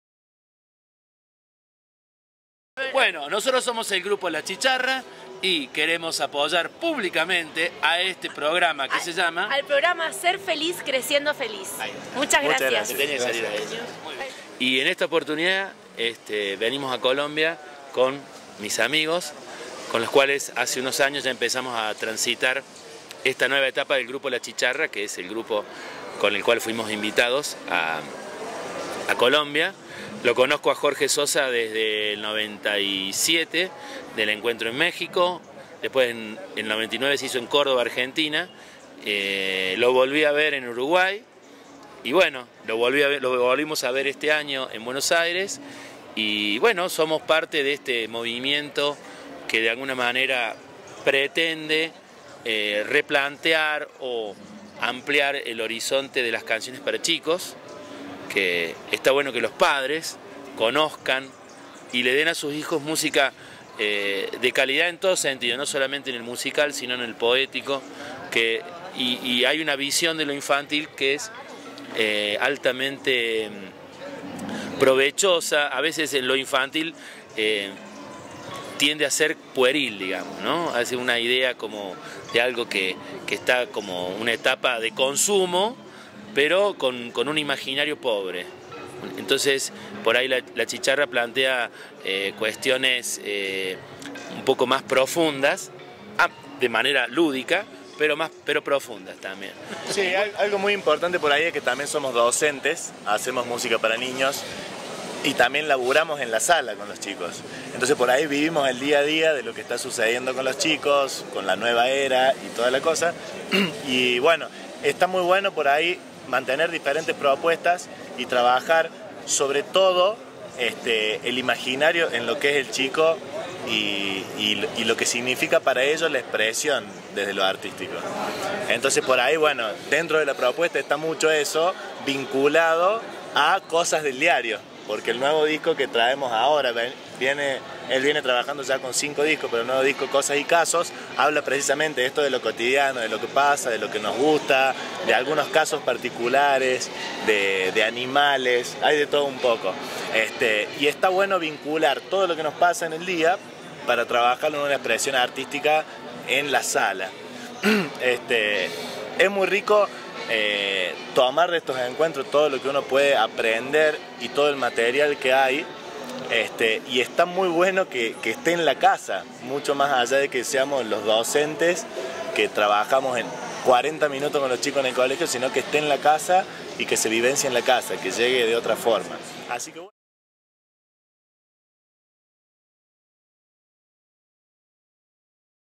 La chicharra apoya la Primera Infancia en Bogotá AIPI - IDARTES
Esta agrupación argentina estuvo en el marco del Undécimo Encuentro de la Canción Infantil Latinoamericana y Caribeña realizada en Bogotá en Septiembre del 2013, envían un mensaje a los niños y sus padres del proyecto AIPI - IDARTES.